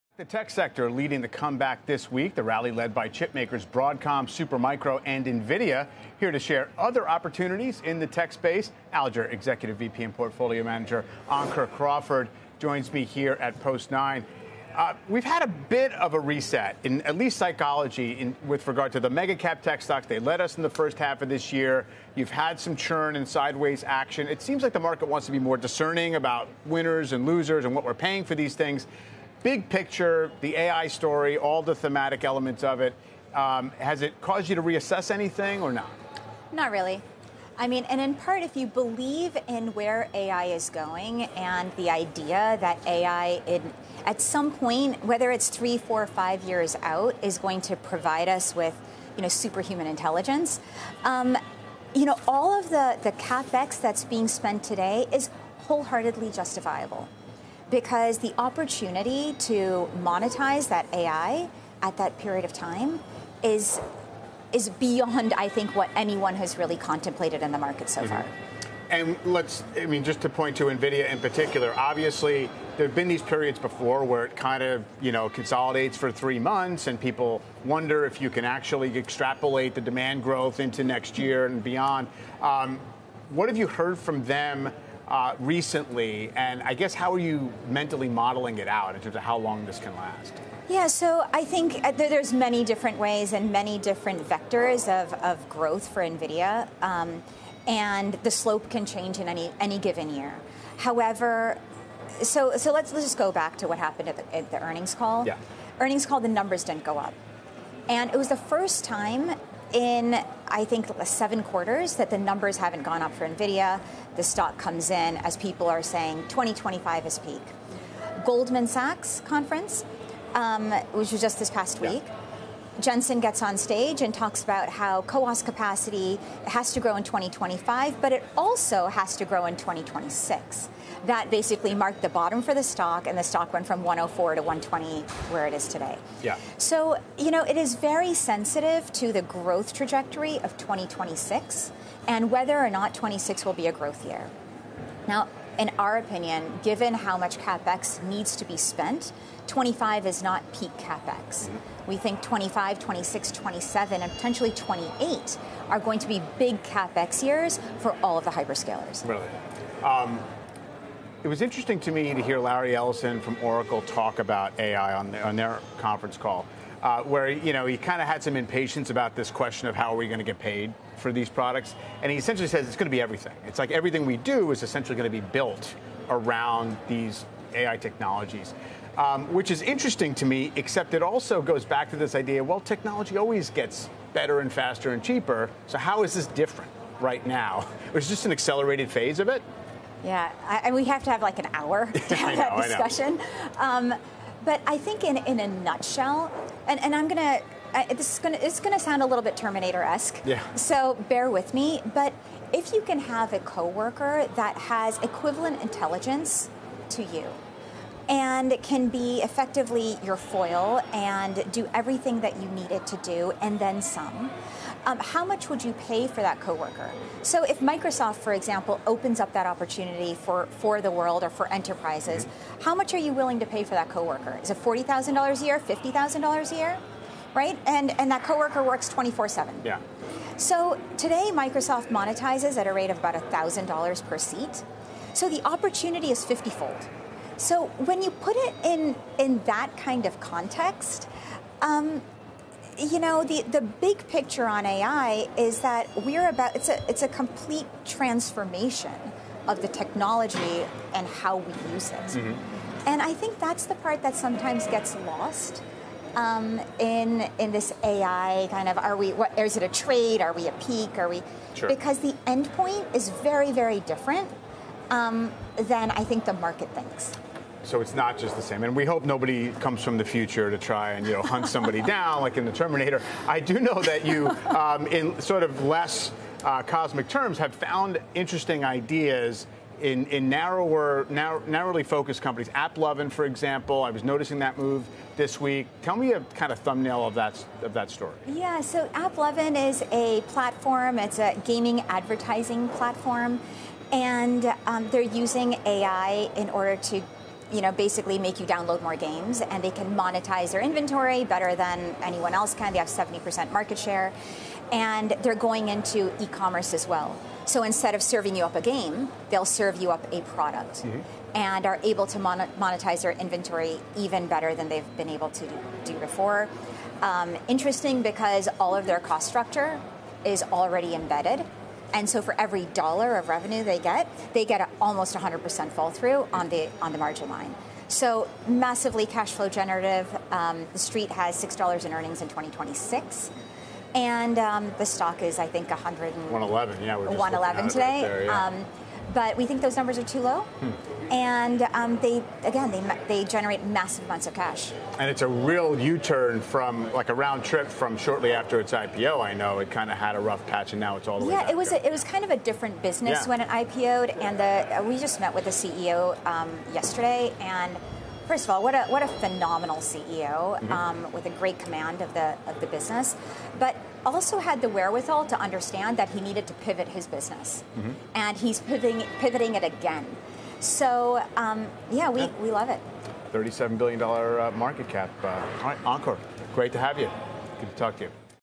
2025-2028 will be big CapEx years for the hyperscalers, says Alger's Ankur Crawford
Ankur Crawford, Alger executive VP, joins 'Closing Bell' to discuss her tech outlook and playbook.